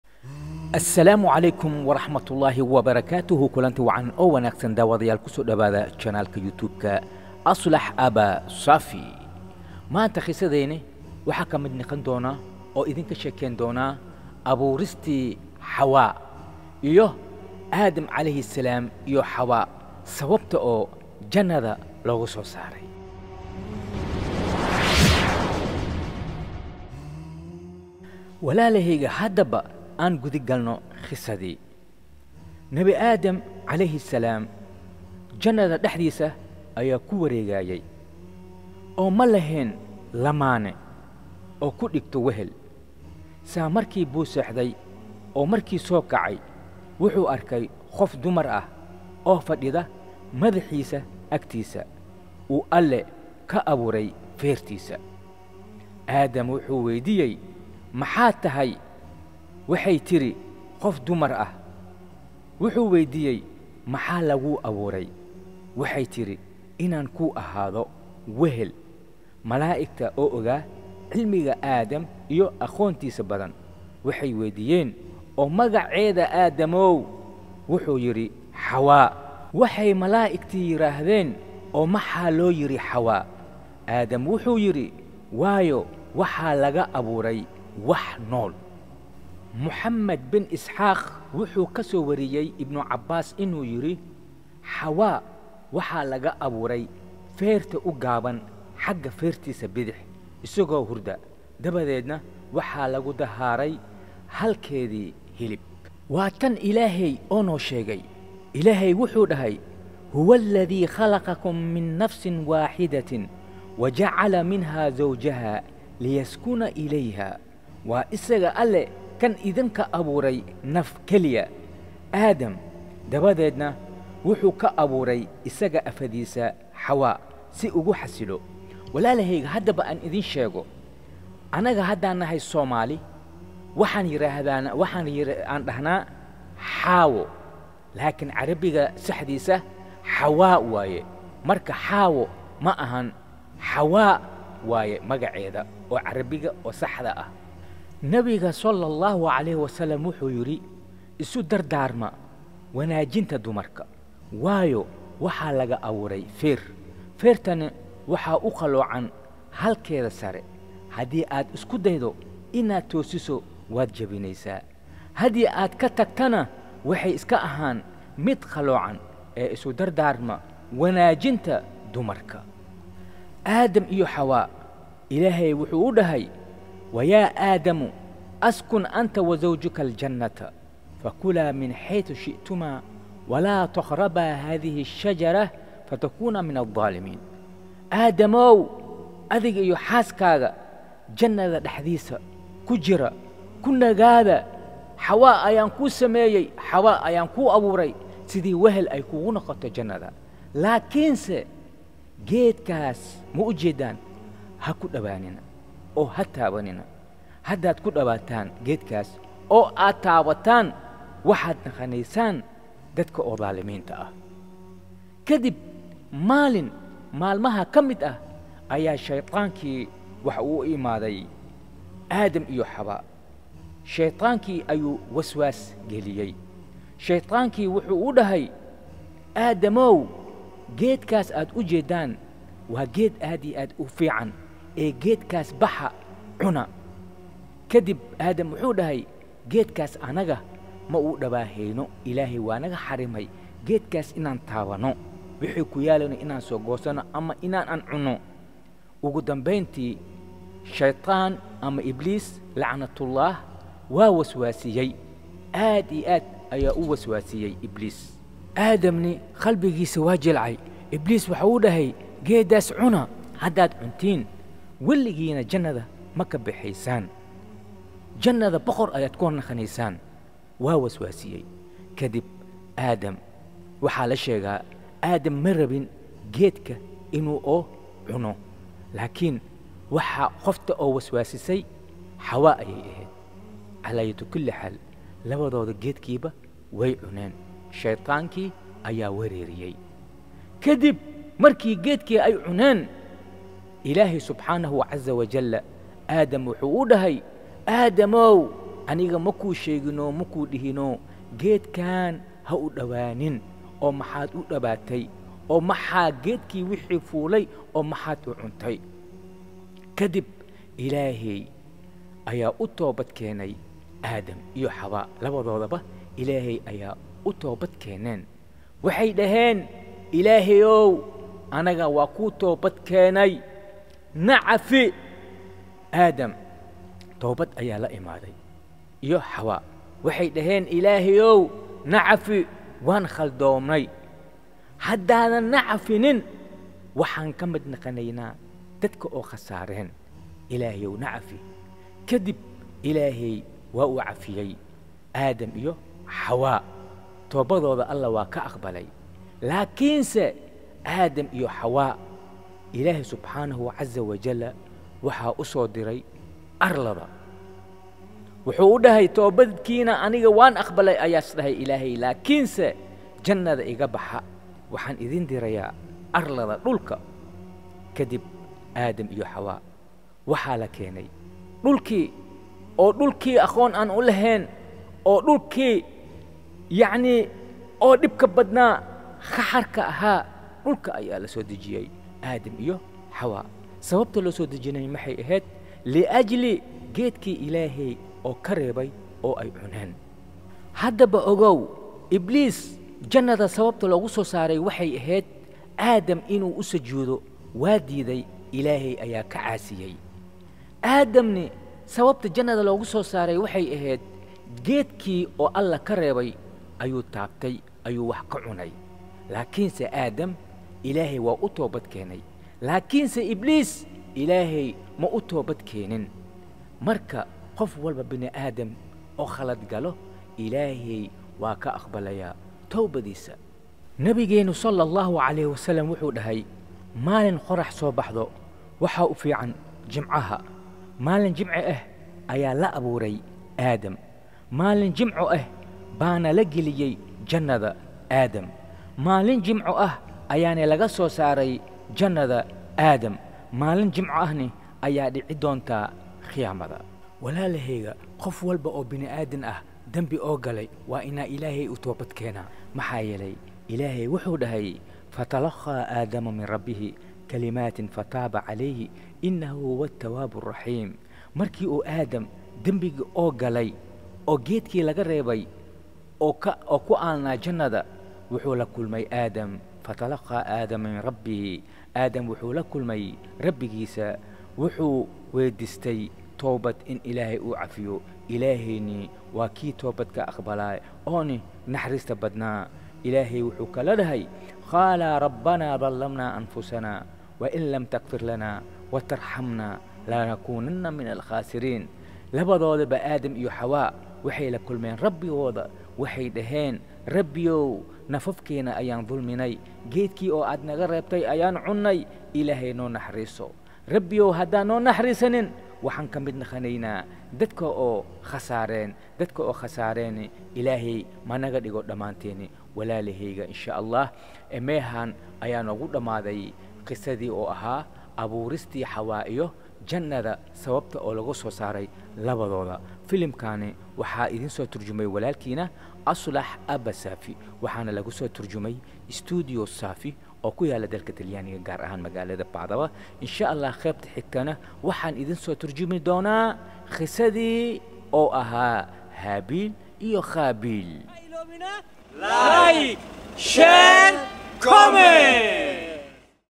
Abuuristii Xawaa Iyo Janada Sababta Loogasoo Saaray
qisadaan waxaa ka hadleeysaa xawaa sida loo abuuray iyo aadam iyo xawaa sababta janada loogasoo saaray